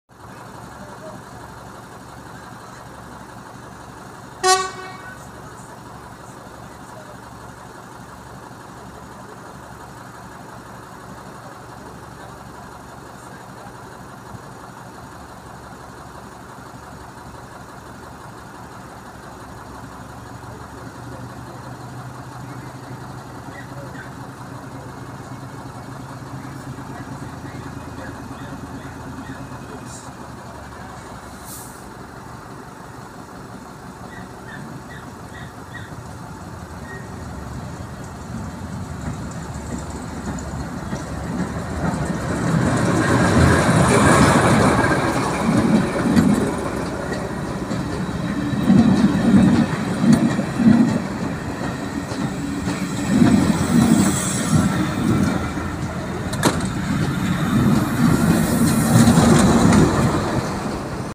PRESERVED CLASS 14.D9523 DEPARTING FROM LEEMING BAR.4 5 24.NEIL HAYTON RAILWAY MEMORIES.
[14.32] DEPARTURE TO SCRUTON.